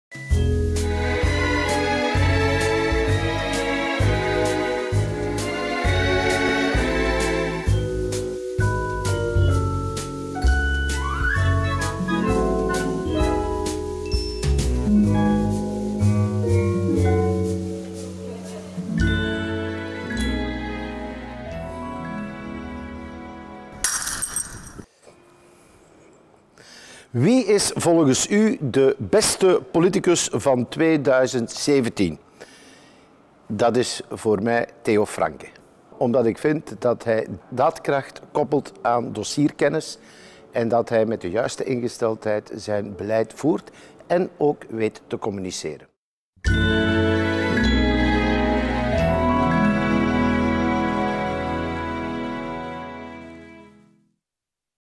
Kerstbalspecial 2017: Kris Van Dijck (N-VA)
De klassieke eindejaarsvraagjes in een feestelijke verpakking: de kerstbalspecial van vlaamsparlement.tv. Vandaag met: Kris Van Dijck van N-VA.